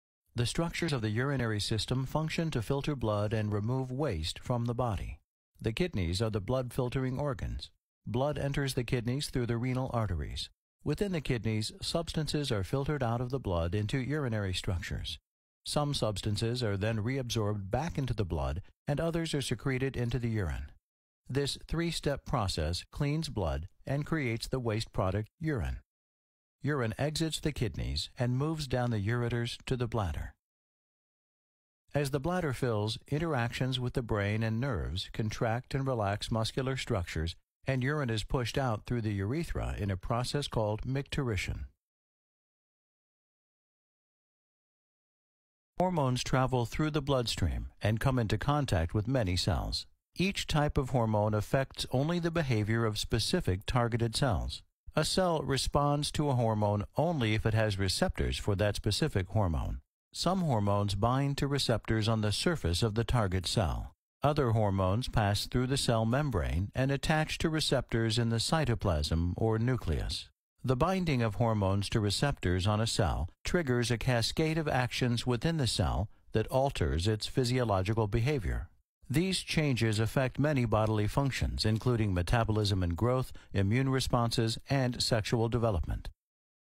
Human Urinary System 3d animation by MWM Medical
The human urinary system consists of the following key components:

1. Kidneys: These bean-shaped organs filter waste and excess substances from the blood to produce urine.

2. Ureters: Tubes that transport urine from the kidneys to the bladder.

3. Bladder: A muscular sac that stores urine until it's ready to be expelled from the body.

4. Urethra: A duct that carries urine from the bladder to the external body, allowing it to be eliminated.

The urinary system plays a vital role in maintaining the body's balance of fluids and electrolytes while removing waste products from metabolism.